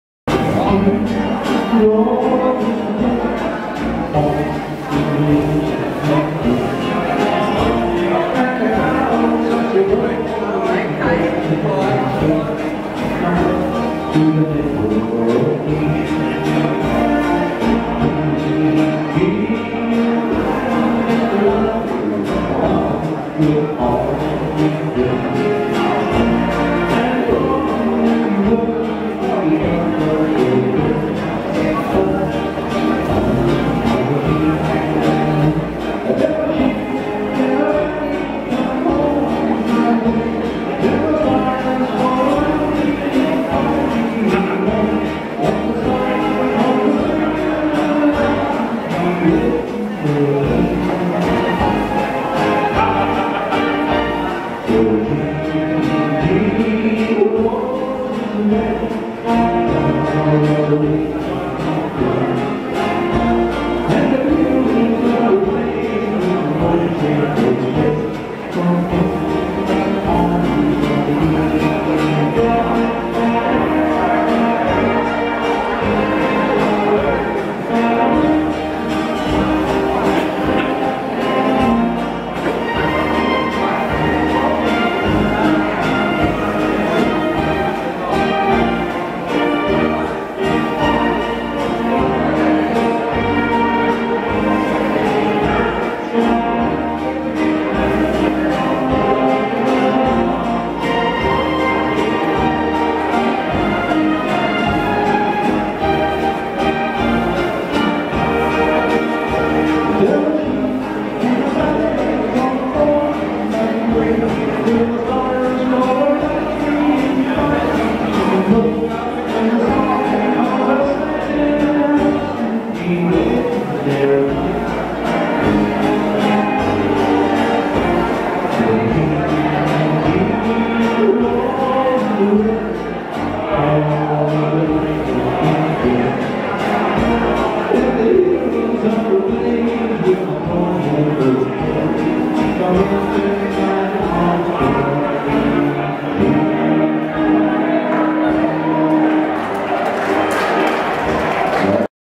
Premier Dance at Warrnambool Saturday Feb 25th 2017
Getaway to the South West Dance Festival  Febrary 2017. Hosted by the Warrnambool and District Dance Club. Dancers enjoying doing the Swing Waltz to the music of Rod McKenzie